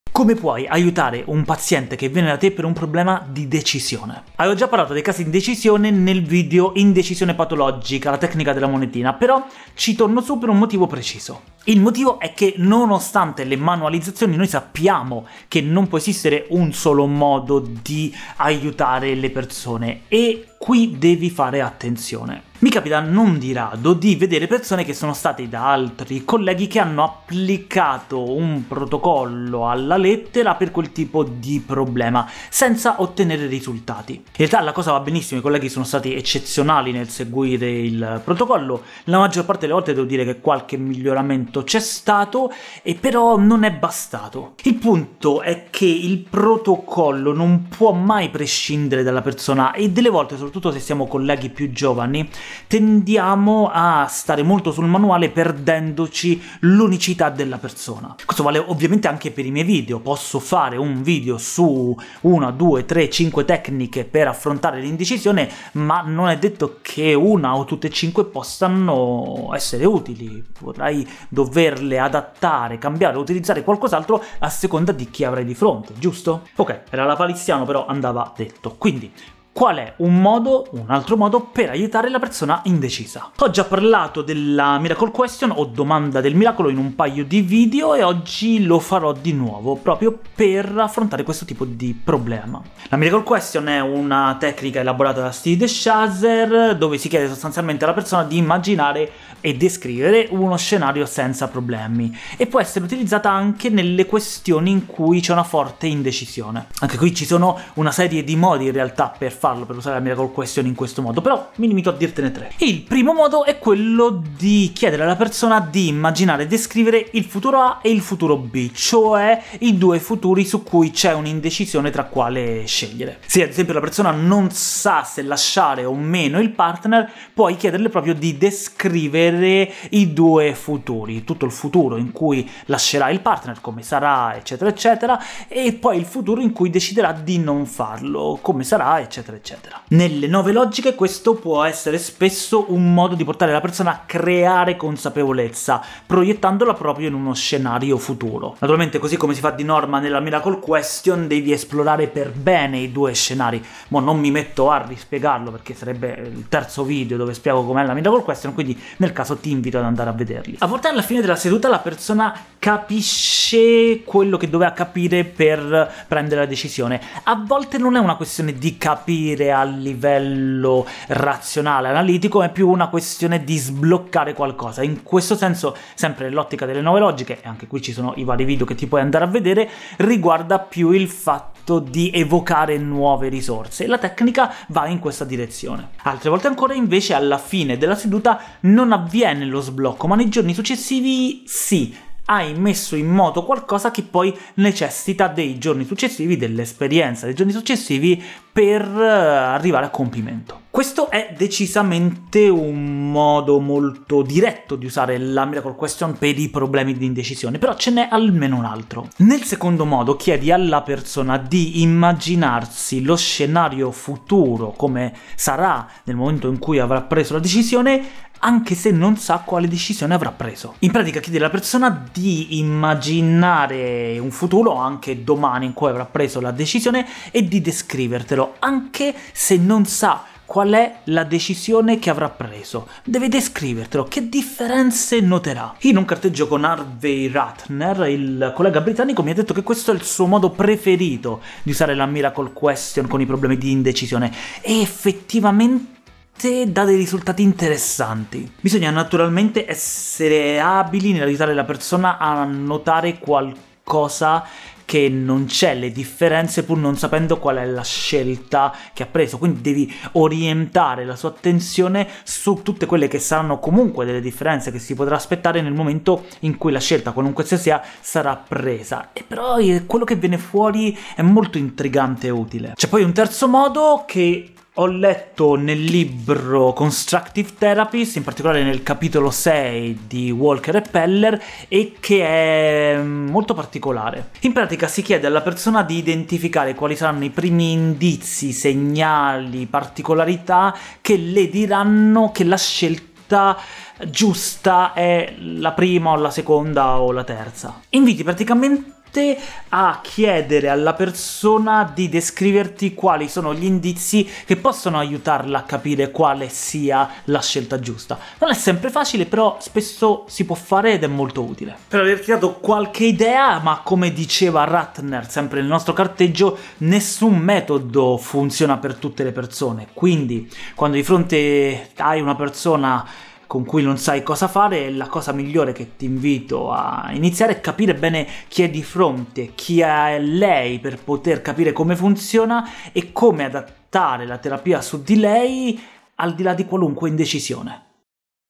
Prendere decisioni con la Miracle Question
La Miracle Question è una tecnica ottima anche per aiutare i pazienti a prendere delle decisioni: scegliere se stare o meno col partner, decidere il proprio futuro lavorativo, scegliere più genericamente tra una soluzione e un'altra, o tra due scelte difficili...

Dopo alcuni video in cui ne parlo più in generale, in questo ne mostro un uso davvero specifico.